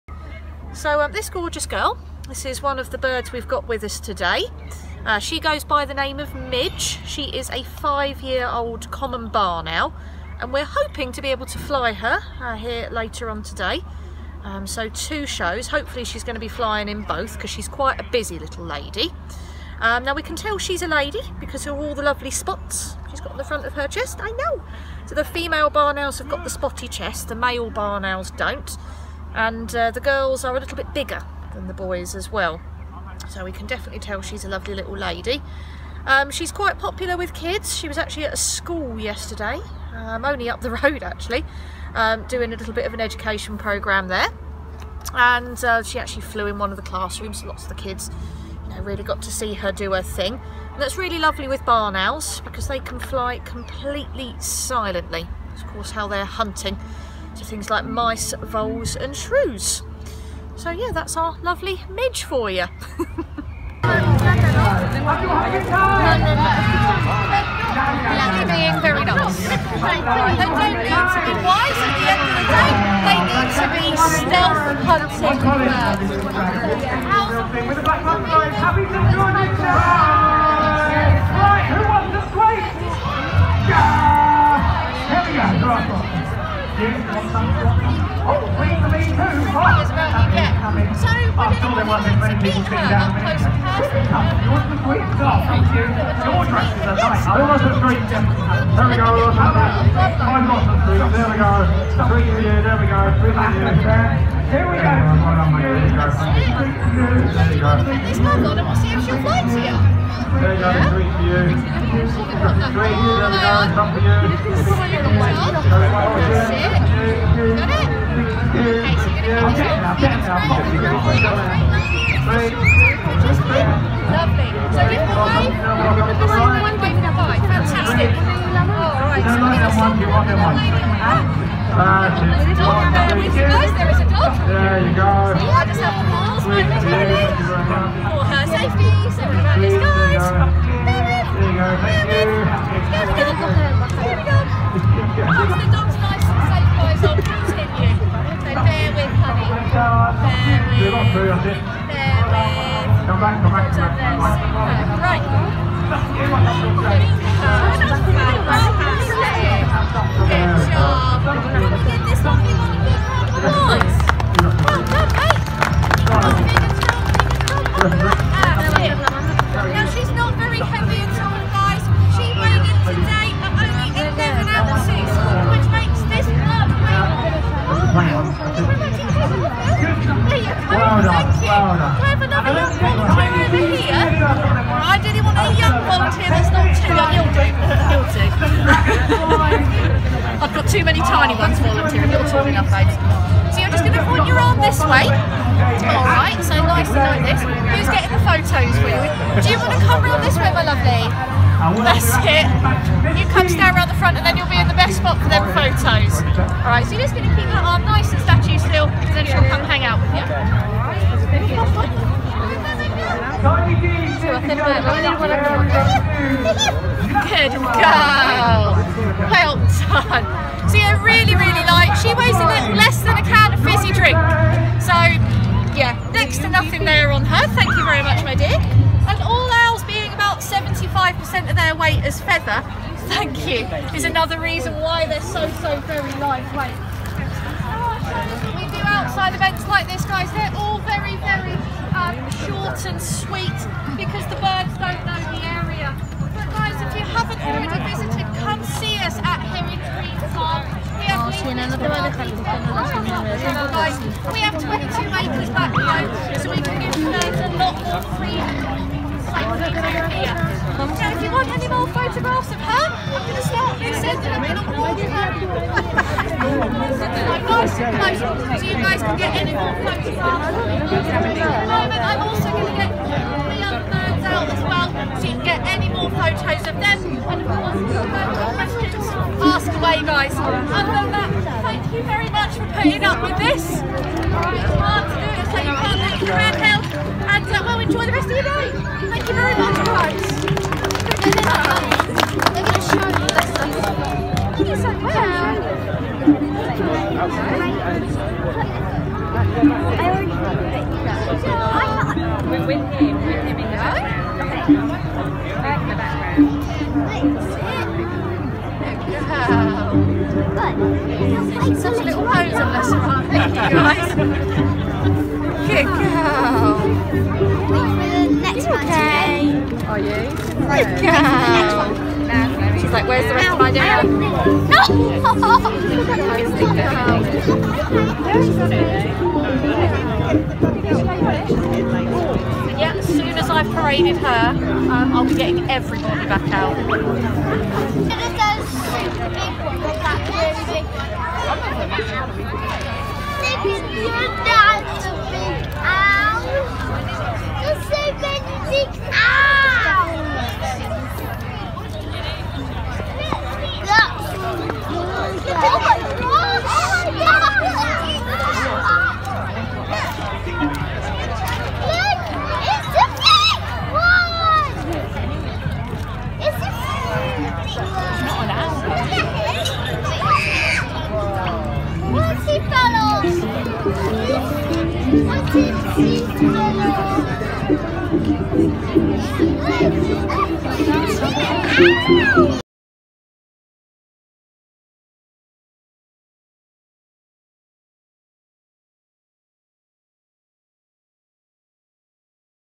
Bird Show At St George’s Day Event In Dunstable 2023
Birds of prey show is back at the priory gardens for the yearly St. George’s day event. 22/4/23

The sound starts off bad but improves during the video, the poor lady that is in charge of the birds turns up every year and ready to share her knowledge with everyone but she is always put somewhere that people can’t hear her, last year she was stuck next to the guy on the chainsaw all day. Maybe next year the bird show could be placed in a quieter area of the park.

Check out the channel for more videos of the  days event